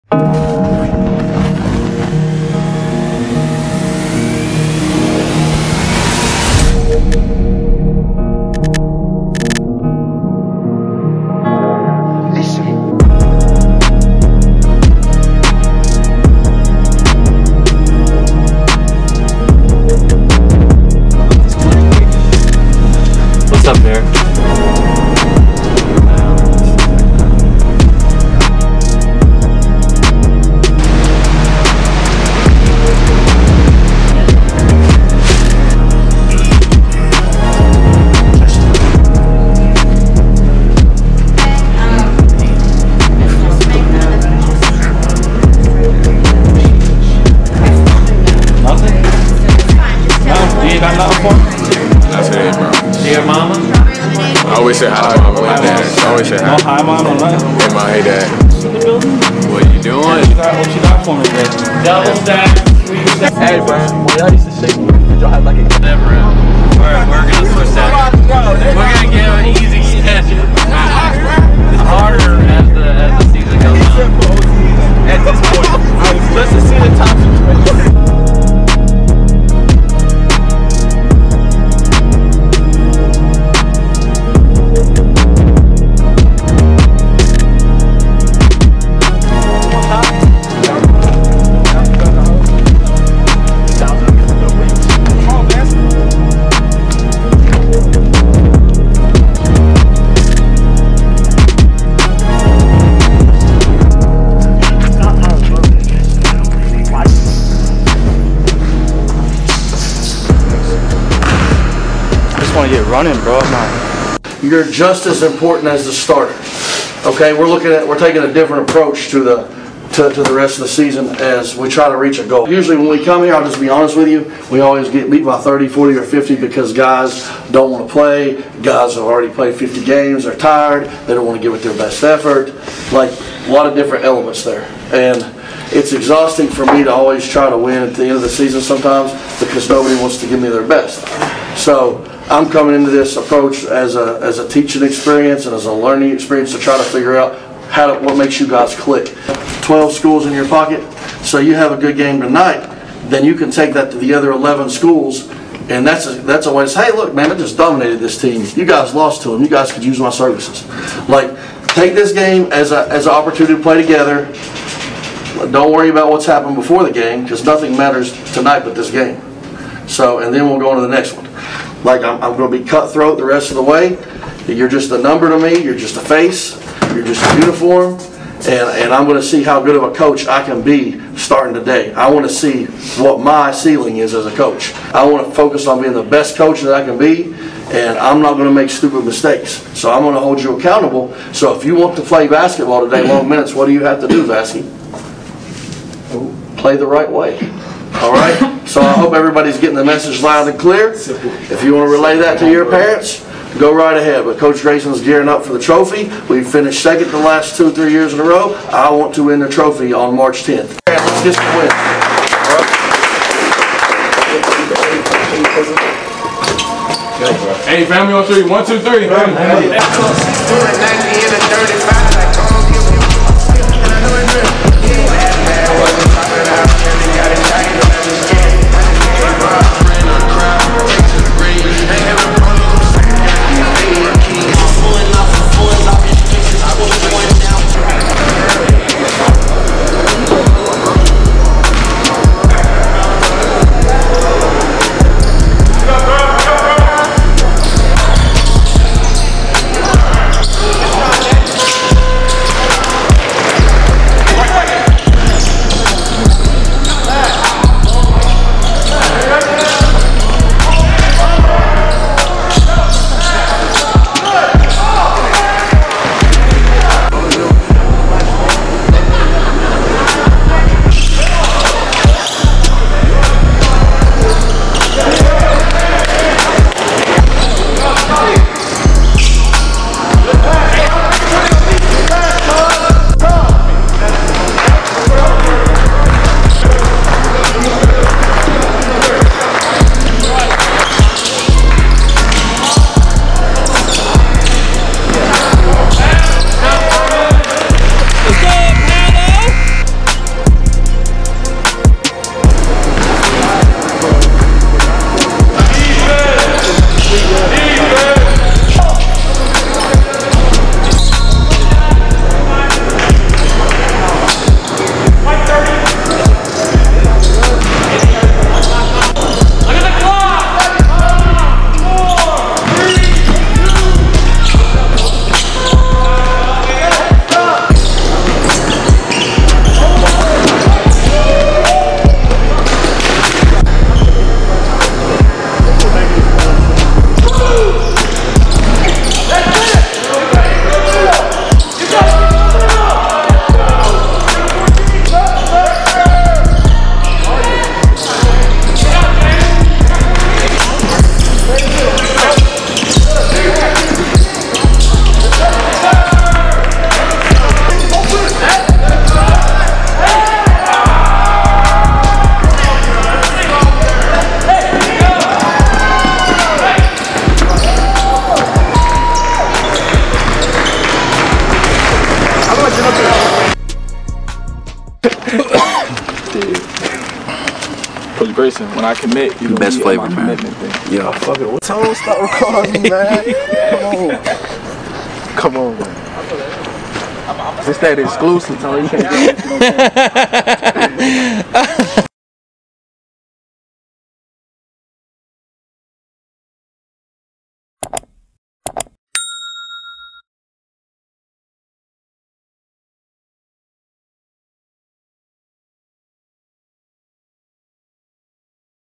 Experience Academy - Episode # 32 (Doc-Series: Season 2): Game Day @ Motlow State CC
Experience Academy Kings Basketball Program kicks off their 7th season. This Episode details the game against Motlow State Community College.

Experience Academy - Season 2 Episode 32 (Kings Will Rise: Doc-Series)

Experience Academy (Episode # 32)
"Kings Will Rise"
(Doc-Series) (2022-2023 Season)

(Directed By: MCS MEDIA GROUP)